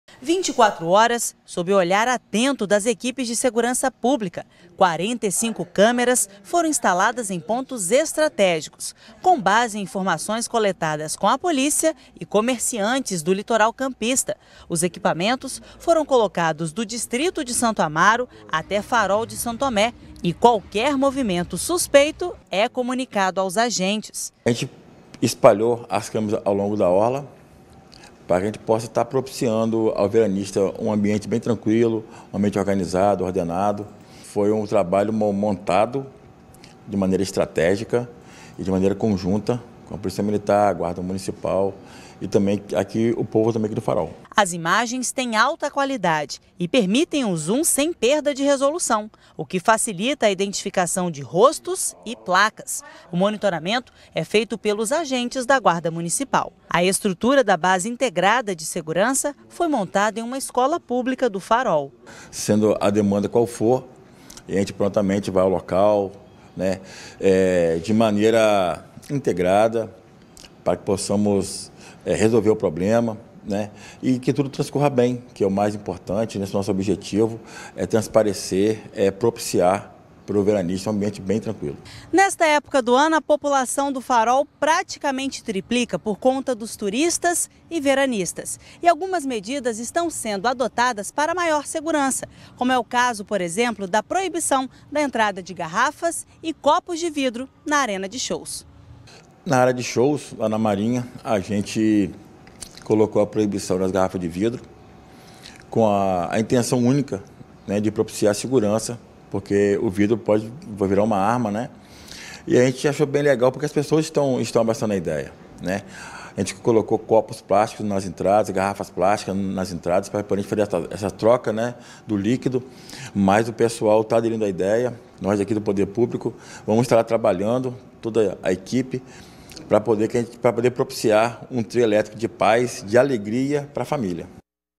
Câmeras de monitoramento reforçam a segurança durante o verão no Farol, em Campos
Para garantir mais segurança durante o verão no Farol de São Tomé, em Campos, câmeras de monitoramento foram colocadas em vários pontos estratégicos ao longo da orla. Na arena de shows novas medidas também foram adotadas.